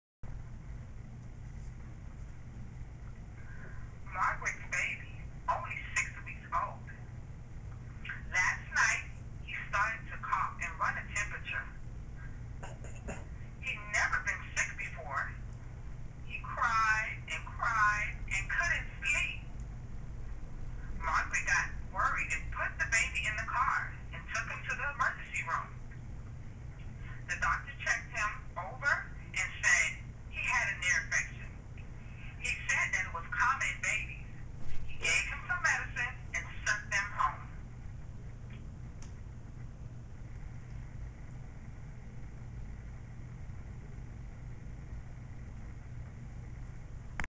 The Babies Sick (Sign Language)
Independent Studies Course at Miami Dade College North Sign Language  Interpretation Program.

Prof. Paula Sargent

Student: Tamiko S.